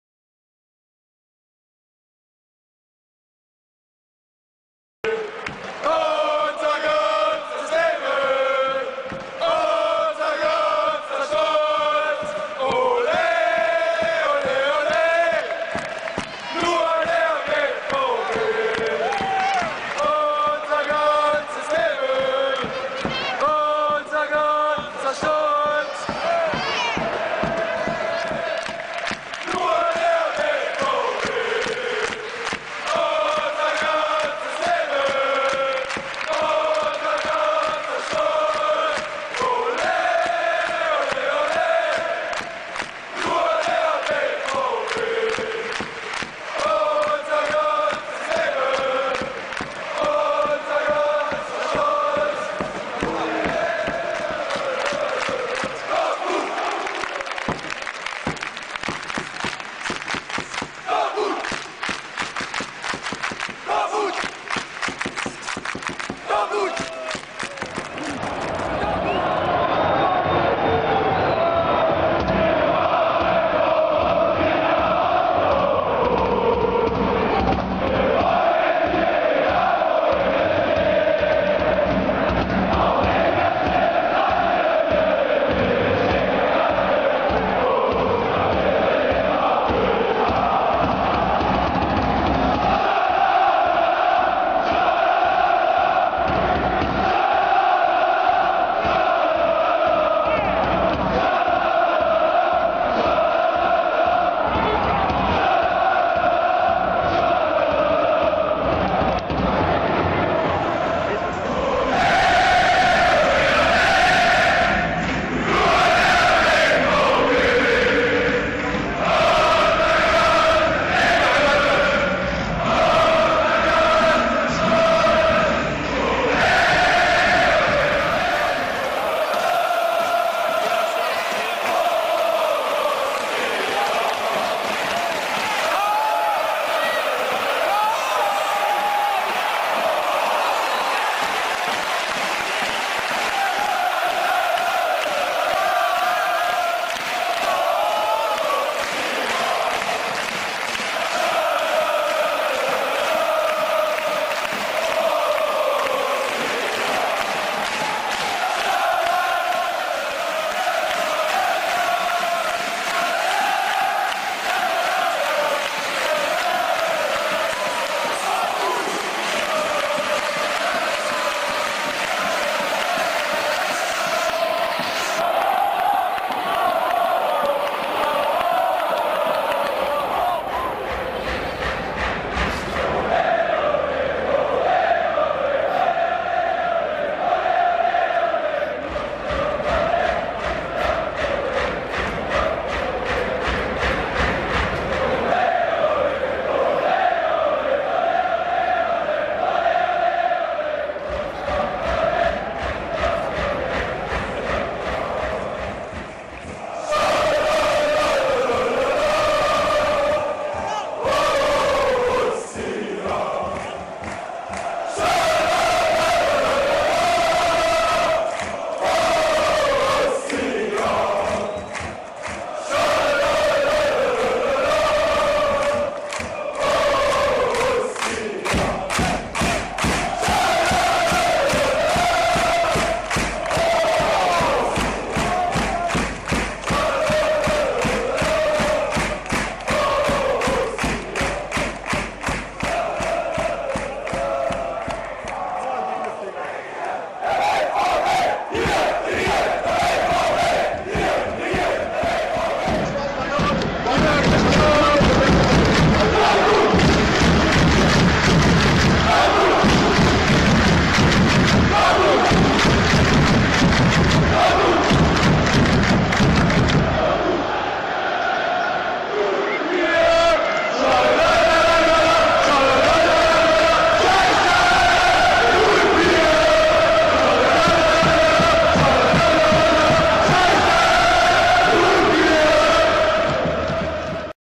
***Exclusic BVB Dortmund Die Meisterfeier
BVB Dortmund s Meisterfeier Atemberaubendes Video Genießen sie diese Augenblicke, unser Dortmund ist deutscher Meister 2010/2011